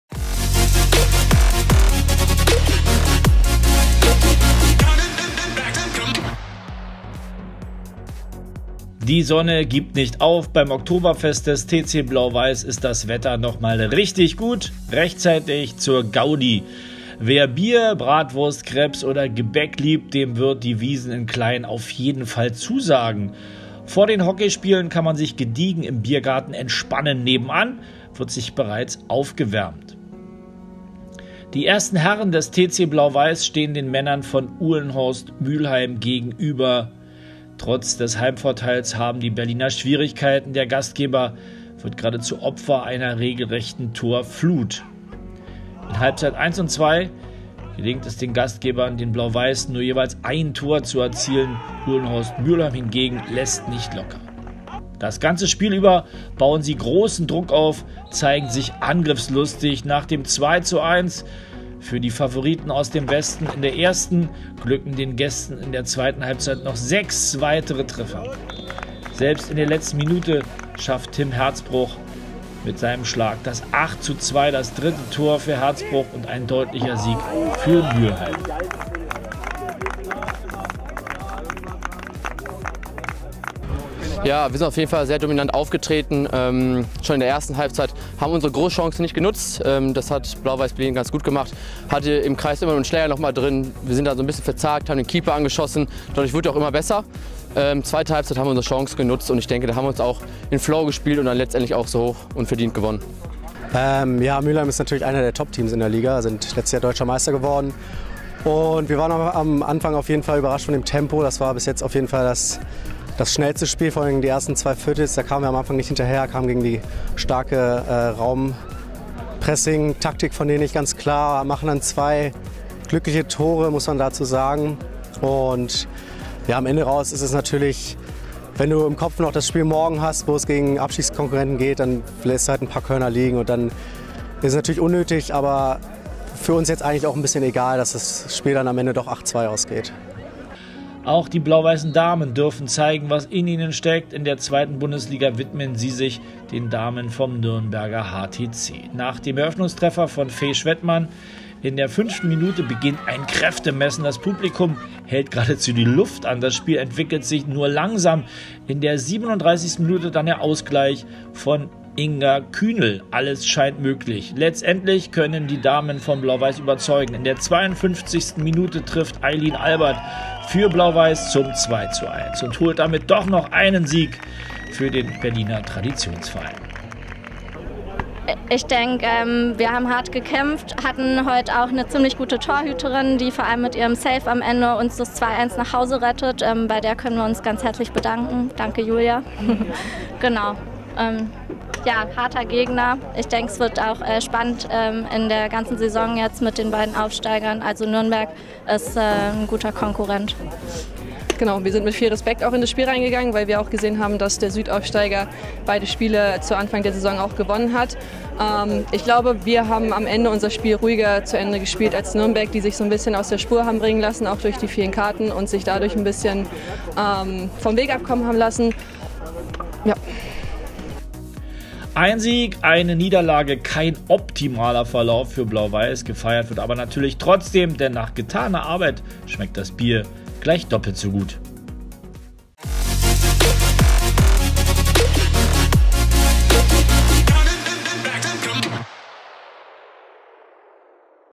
Hockey Doppelpack beim TC Blau Weiß 1899 | 1.+ 2. Hockey-Bundesliga
Gaudi beim TC Blau Weiß 1899 | Hockey Doppelpack

Neben dem Hockey, kann man beim Berliner Traditionsverein über die hauseigene Wiesn laufen und ein Bier in der Sonne genießen. Die Herren standen derweil in der 1. Hockey-Bundesliga gegen den Meister 2017 Uhlendorf Mülheim auf dem Feld. Die Damen empfingen in der 2. Hockey-Bundesliga die Damen des Nürnberger HTC ...